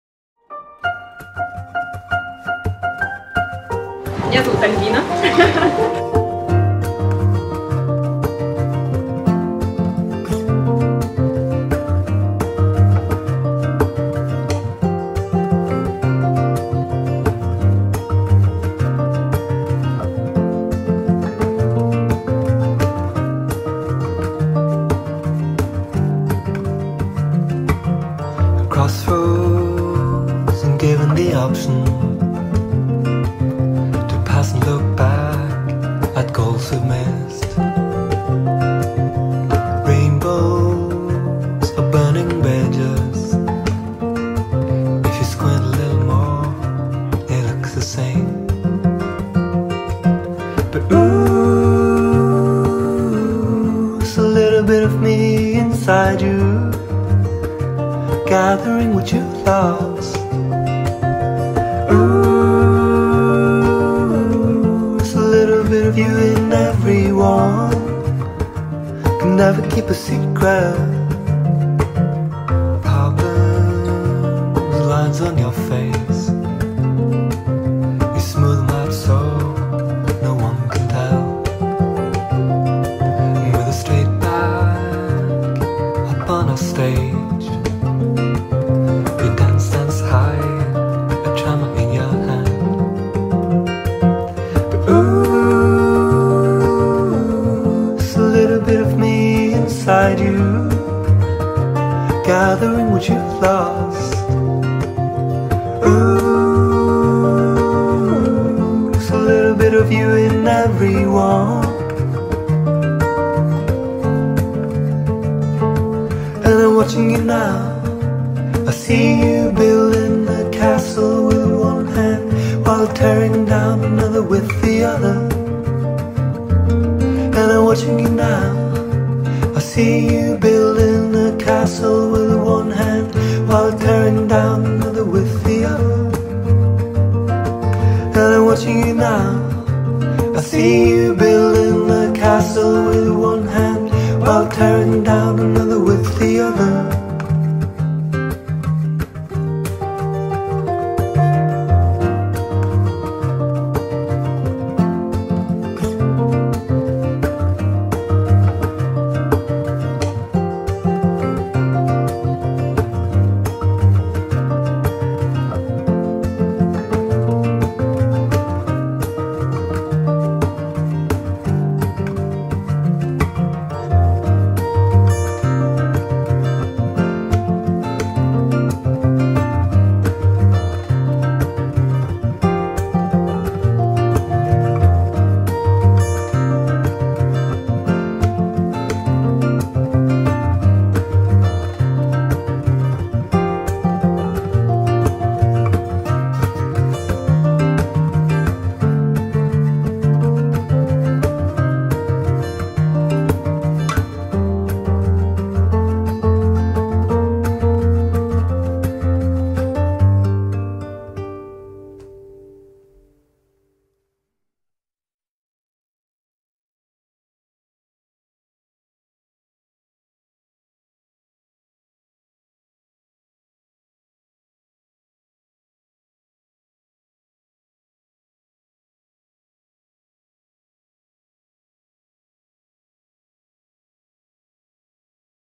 "Flower Power" Workshop
Цветочный воркшоп студии флористики "Flower Power", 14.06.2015. Место: "Простобар Еда"